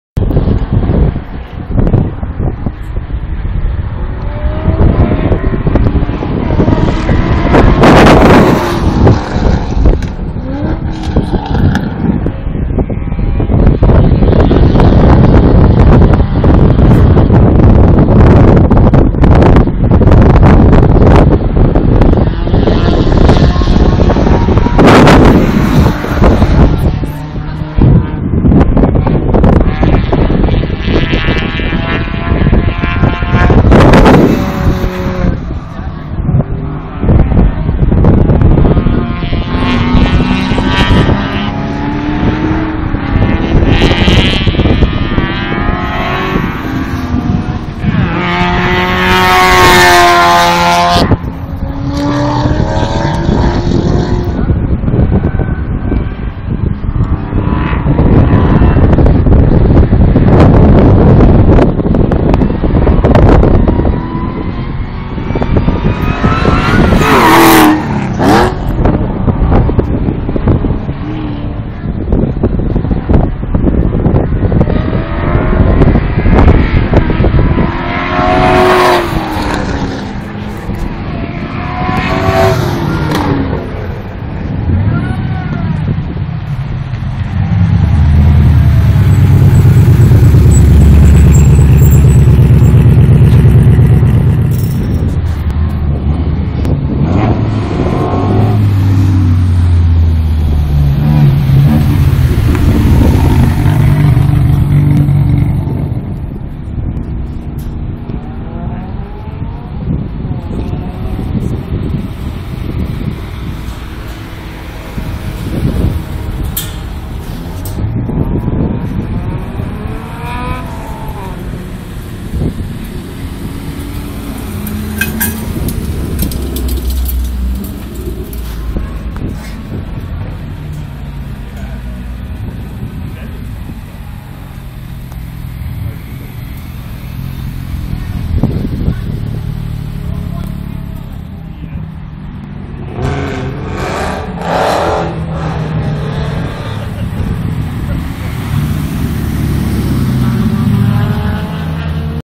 Time Attack 5 - Group B
Crappy phone video of group B doing a quick fly-by, followed by some of the vehicles coming off the track via the pit-lane.